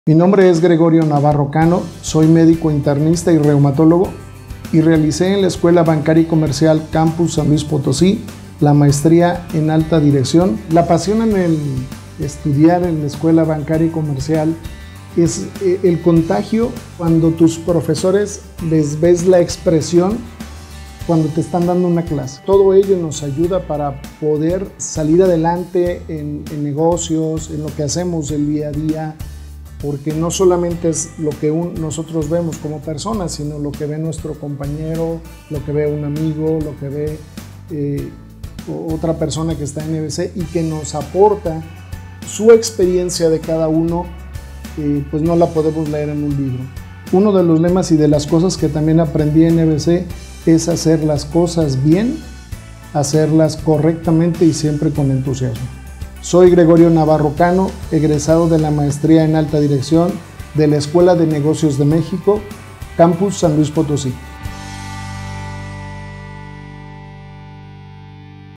Dr. Gregorio Navarro Cano, egresado de la Maestría en Alta Dirección | EBC Posgrados
En palabras del Dr. Navarro Cano, "algo que aprendí en la EBC es hacer las cosas bien y siempre con entusiasmo", de ahí que estudiar este posgrado haya significado una experiencia enriquecedora en favor de su crecimiento profesional.